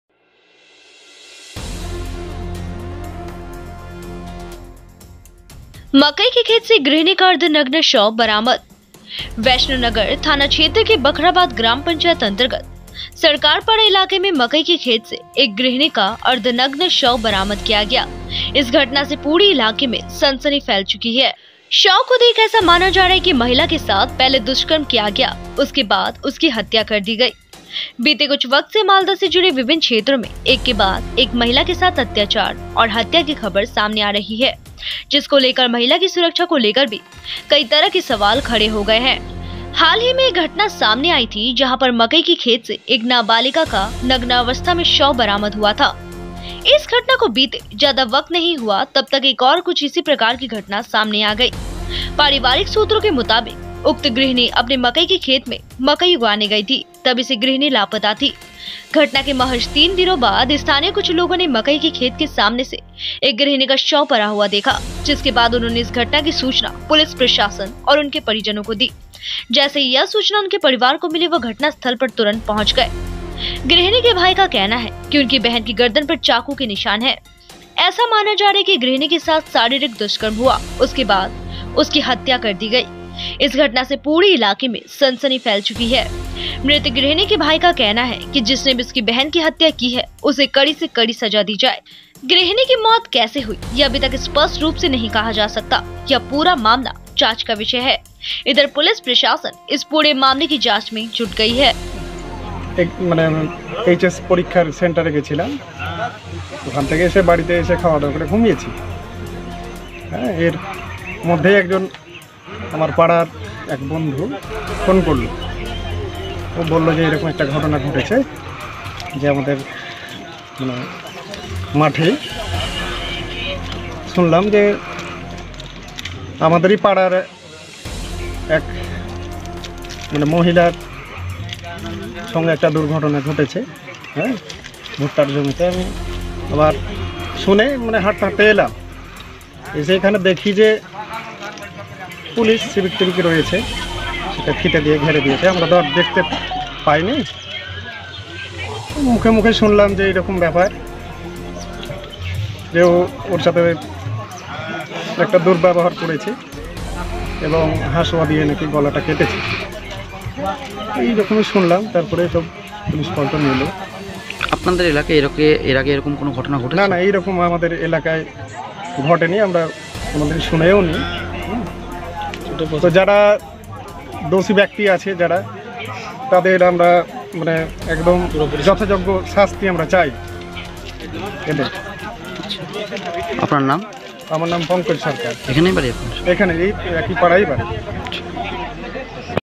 Dead body of a house wife recovered from the corn field; Police is investigating the matter
Kaliachak: Dead body of a house wife recovered from the corn field;  Police is investigating the matter